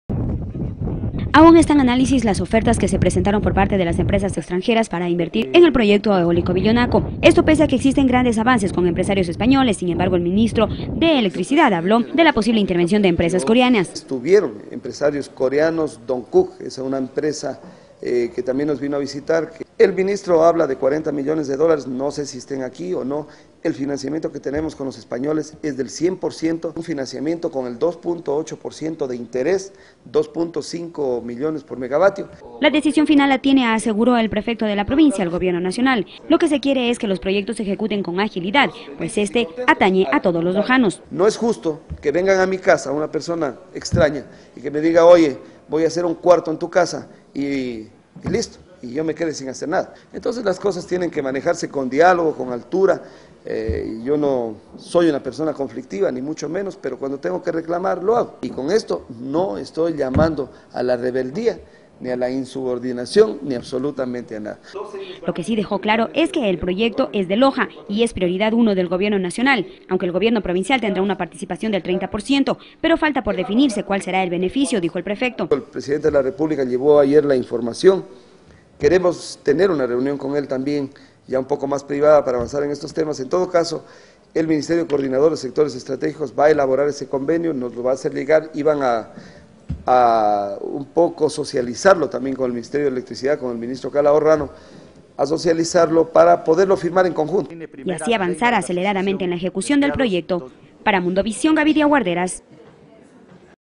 Según Prefecto, la mejor propuesta será la que lleve a cabo el proyecto eólico
La mejor propuesta que se presente a nivel internacional será la que se escoja para llevar adelante el proyecto eólico Villonaco, dijo el prefecto de la Provincia, Rubén Bustamante, esto tras las declaraciones del ministro de Electricidad, quien señaló que serán empresarios coreanos los que estén frente al proyecto.